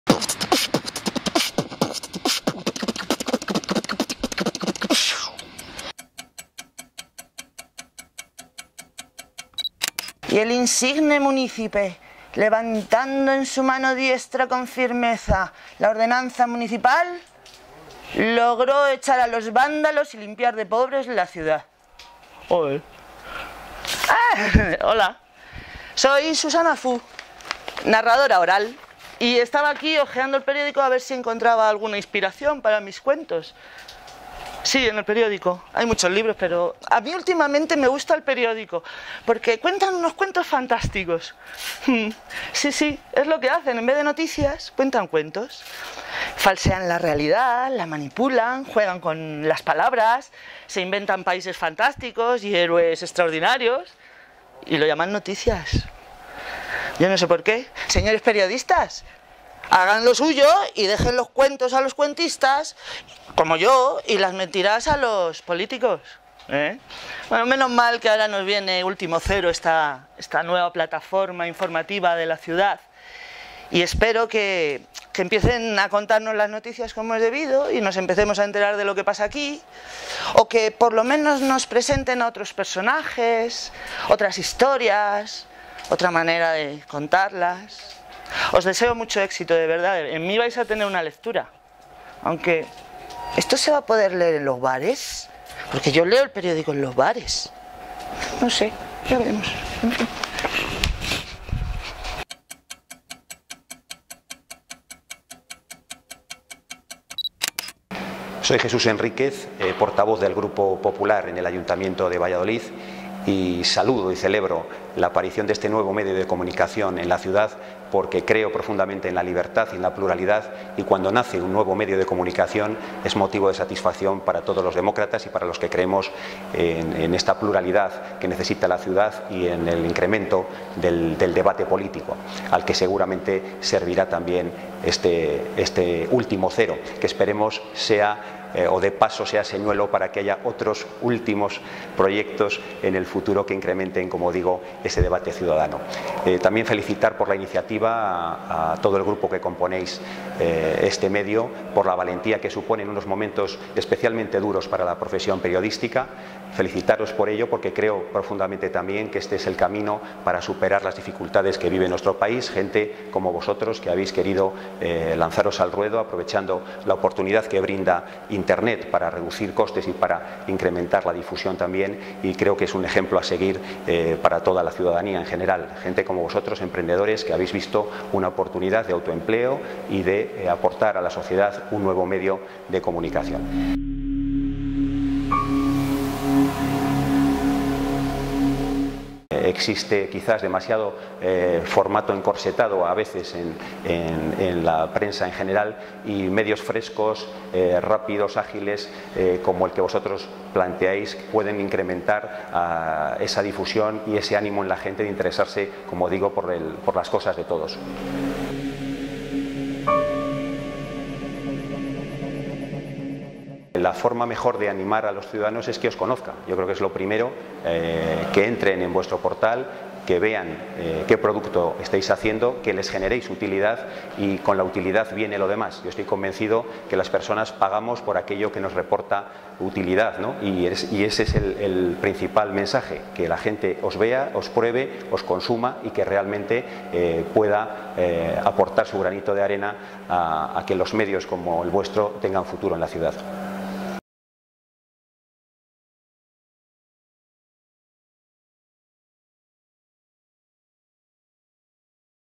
Susana Fú y Jesús Enríquez - últimoCero (27/11/2012)
Susana Fú, narradora oral, y Jesús Enríquez, portavoz del PP en el Ayuntamiento de Valladolid, protagonizan este vídeo de apoyo a últimoCero